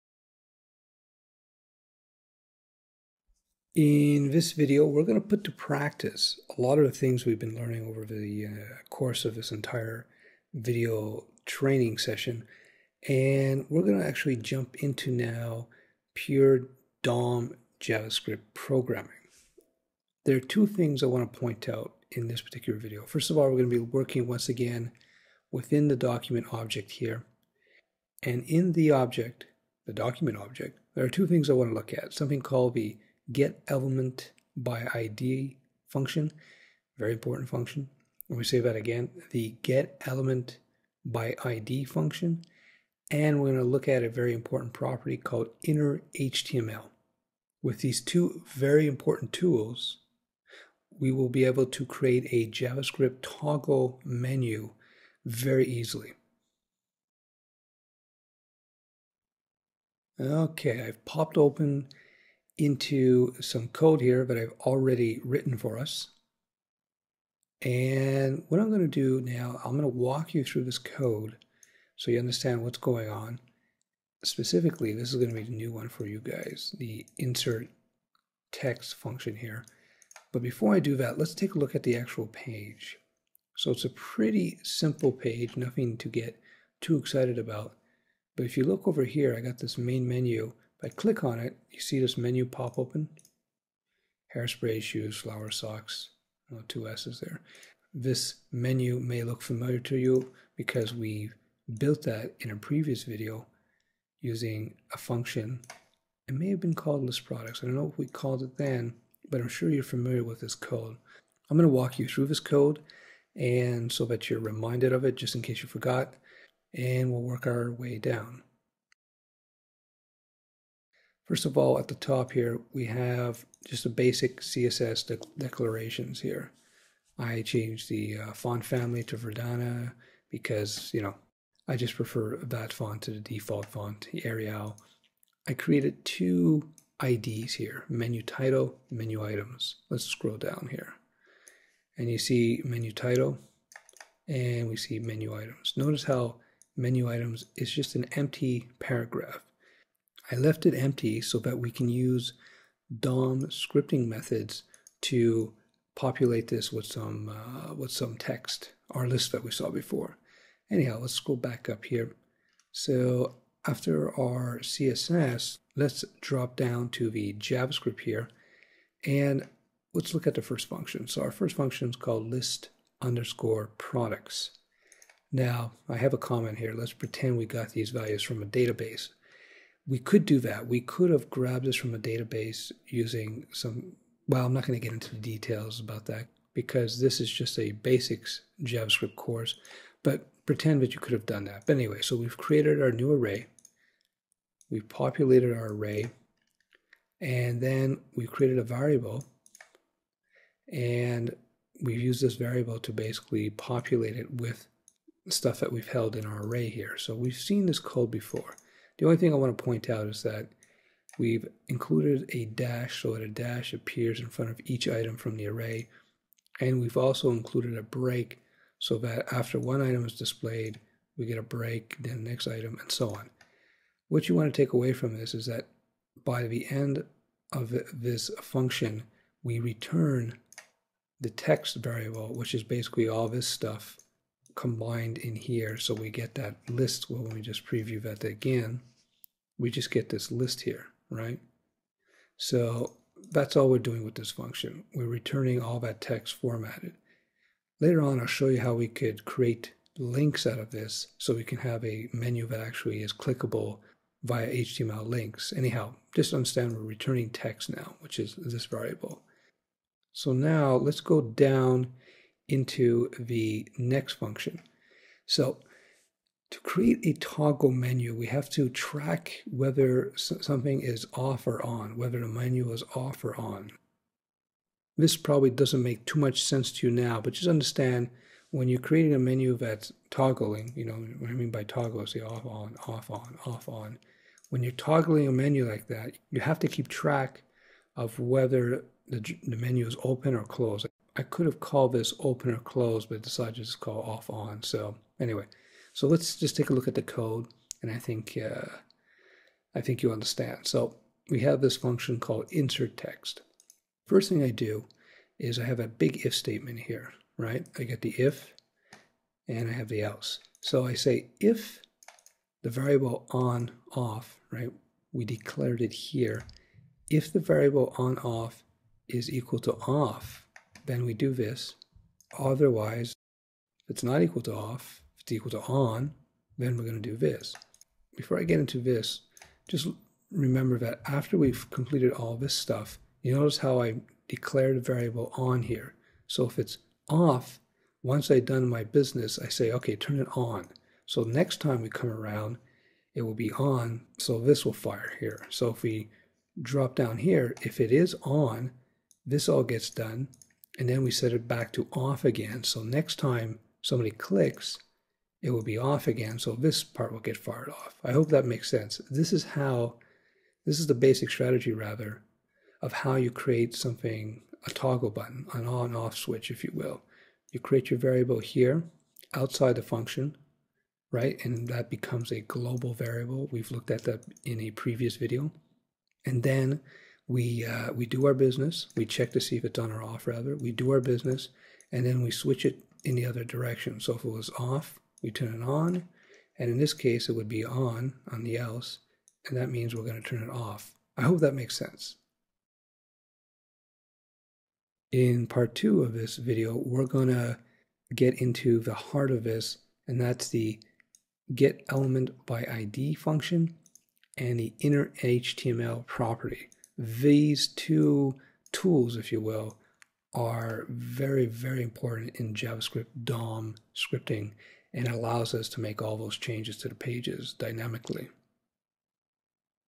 Beginner's Javascript - Toggle Menu Part 1
This is part 1 of a 2 part video tutorial on creating the Javascript toggle menu, from our Beginner's Javascript course.